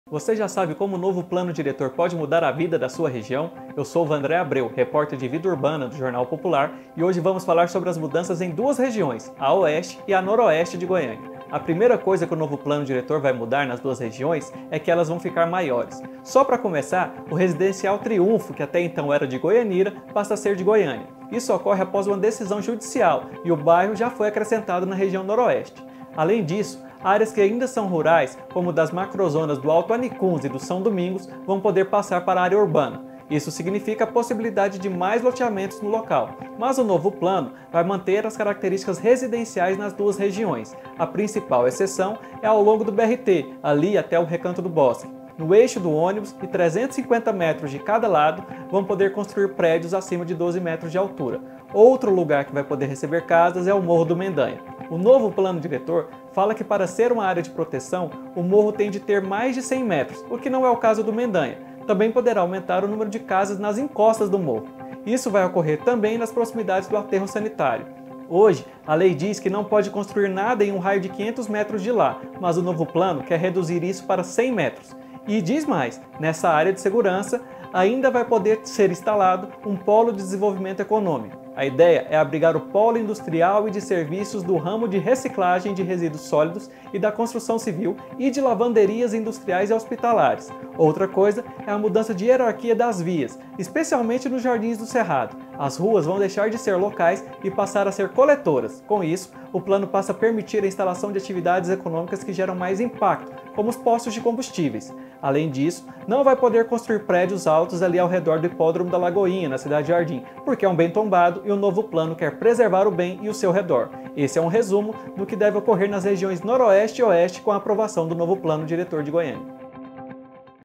PLANO DIRETOR DE GOIÂNIA | O que vai mudar nas regiões Oeste e Noroeste?
Nesta semana O POPULAR apresenta os impactos do novo Plano Diretor de Goiânia em cada região da cidade. Acompanhe em opopular.com.br a reportagem completa sobre como as regiões Oeste e Noroeste serão impactadas pelas novas determinações do Plano.